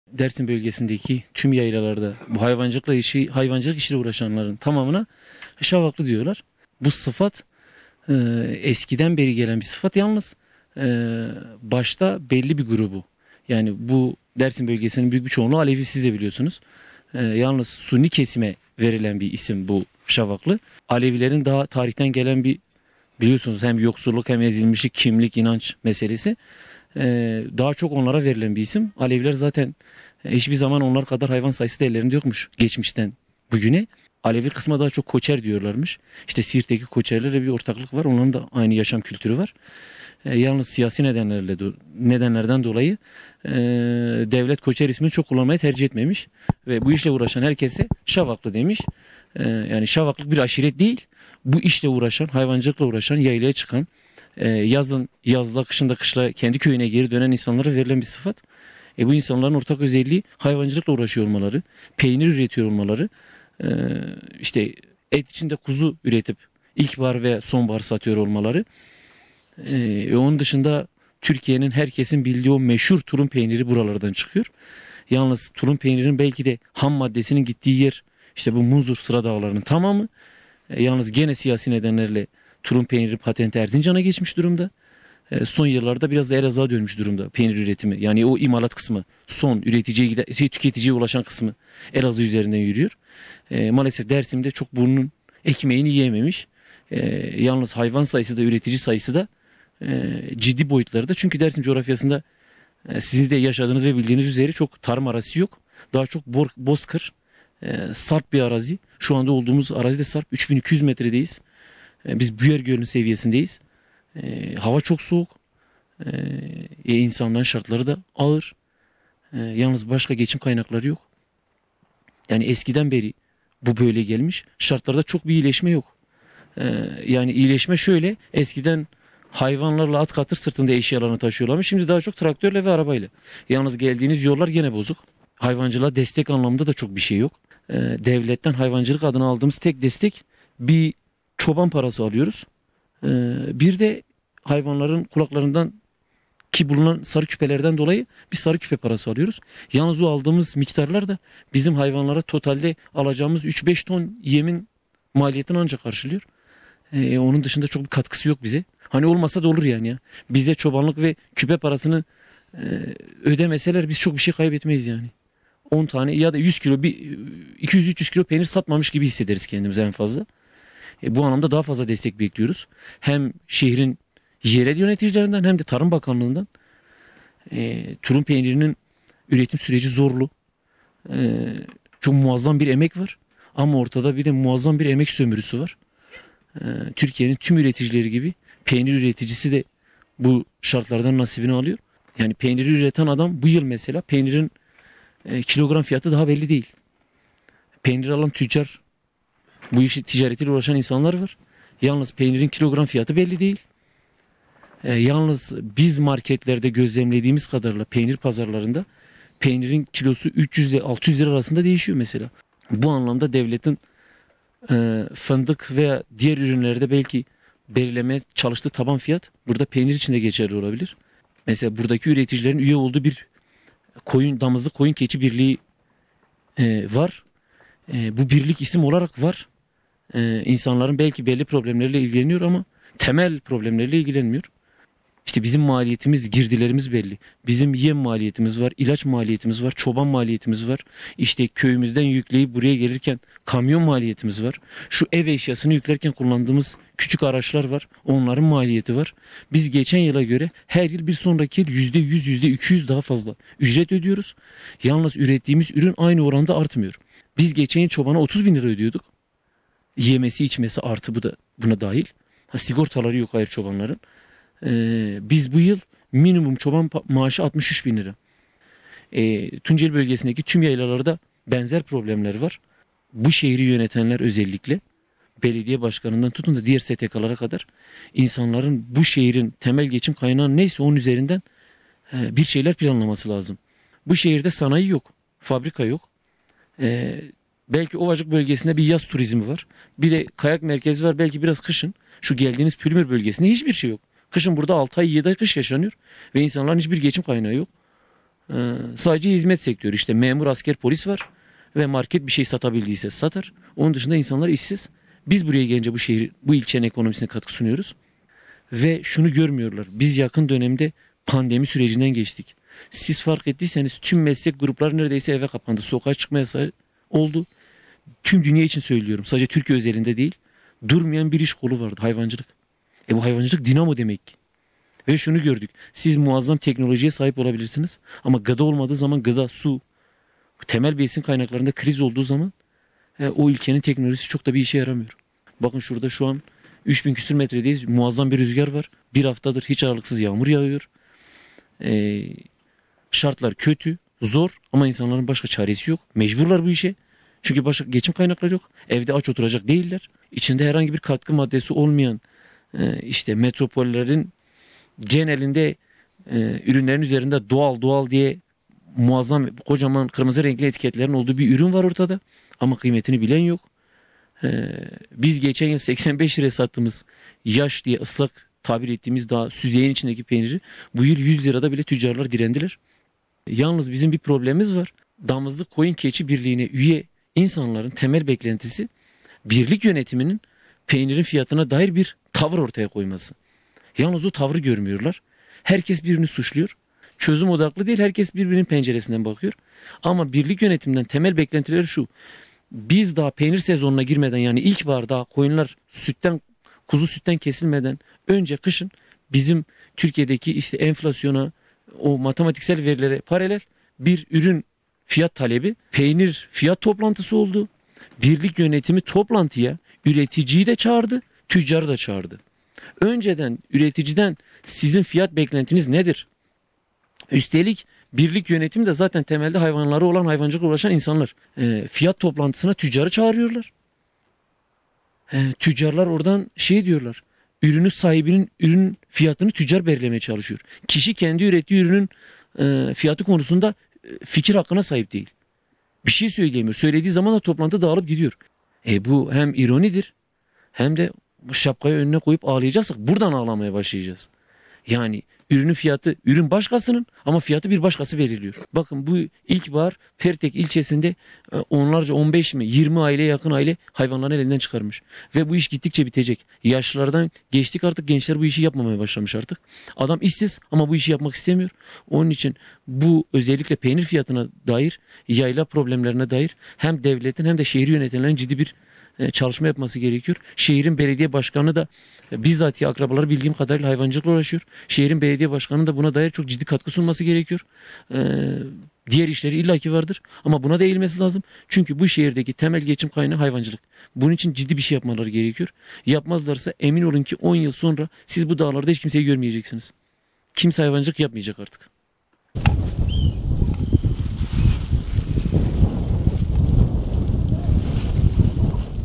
Dersimli yaylacılar dertli: Böyle giderse 10 yıl sonra bu dağlarda kimseyi göremezsiniz
PİRHA – Pülümür’ün 3000 rakımlı Hel Dağı’nda yaylacılara konuk olduk. PİRHA’ya konuşan yaylacılardan Zeki Çelebi, “Pertek’te 20’ye yakın aile hayvancılığı bıraktı. Bu iş gittikçe bitecek. Böyle giderse, emin olun ki 10 yıl sonra siz bu dağlarda hiç kimseyi göremeyeceksiniz. Kimse hayvancılık yapmayacak artık” dedi.